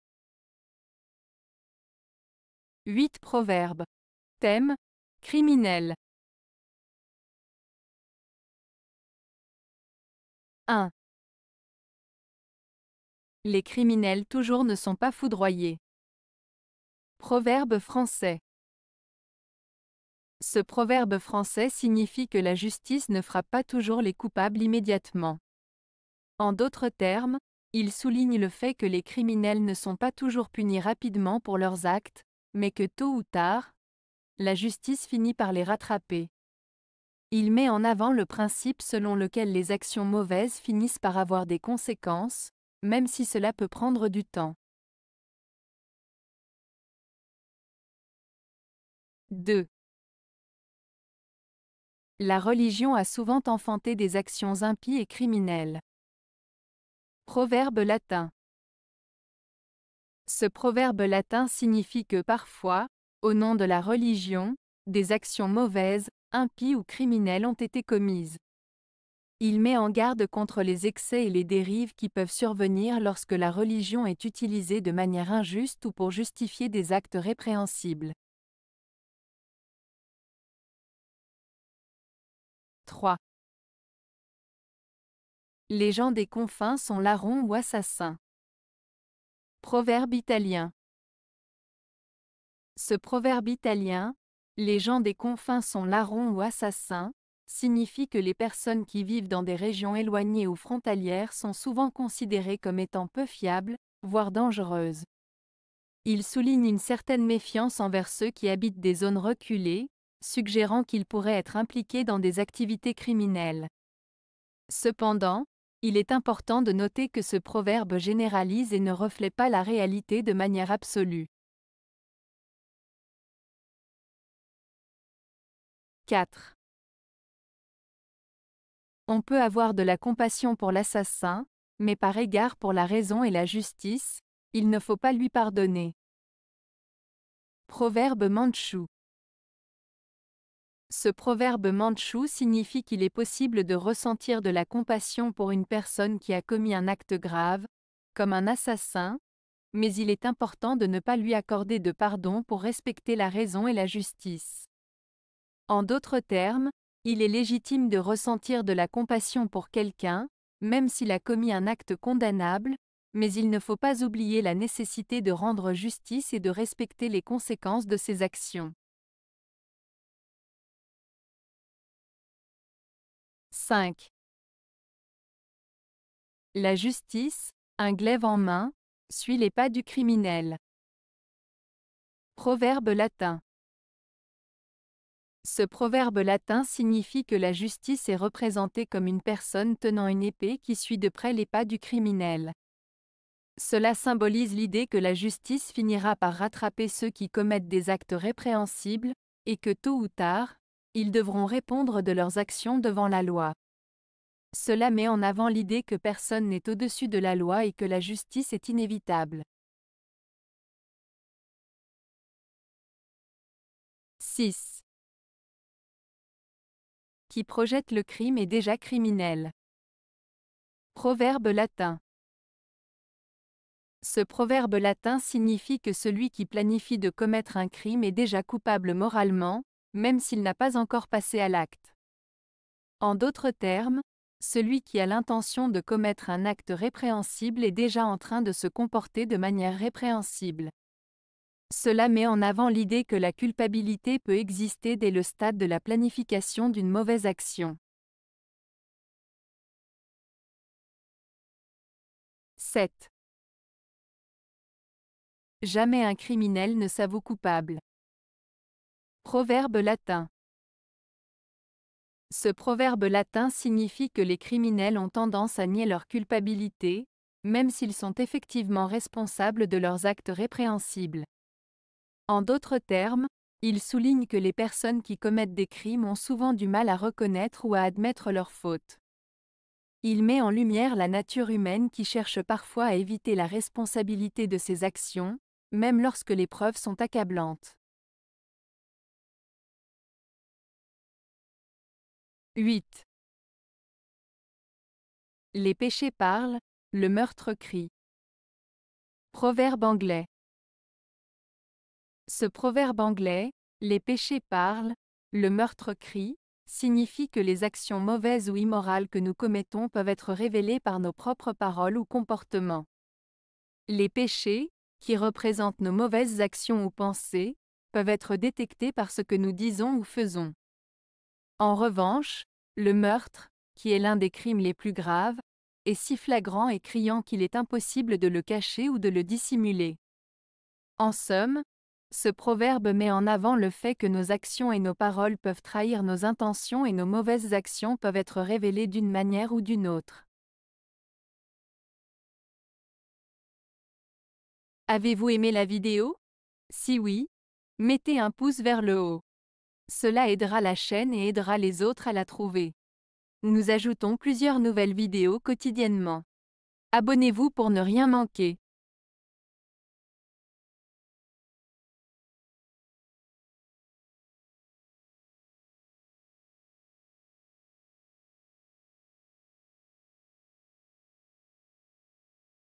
Proverbes - Criminel (2 de 2)
00:00 Introduction
00:08 Proverbe Français - Les criminels toujours ne sont pas foudroyés.
00:46 Proverbe Latin - La religion a souvent enfanté des actions impies et criminelles.
01:20 Proverbe Italien - Les gens des confins sont larrons ou assassins.
02:03 Proverbe Mandchou - On peut avoir de la compassion pour l'assassin, mais par égard pour la raison et la justice, il ne faut pas lui pardonner.
02:51 Proverbe Latin - La justice, un glaive en main, suit les pas du criminel.
03:31 Proverbe Latin - Qui projette le crime est déjà criminel.
04:10 Proverbe Latin - Jamais un criminel ne s'avoue coupable.
04:50 Proverbe Anglais - Les péchés parlent, le meurtre crie.